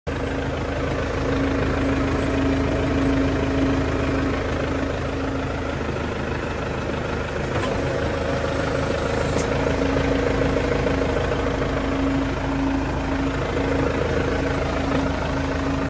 JOHN DEERE 6300 For Sale
John Deere 6300 tractor runs out good cables for shifting need some attention. Hydraulics to curl the bucket are a little slow but do work. Tractor came off an estate located in sterling IL, for information call Adam Almburg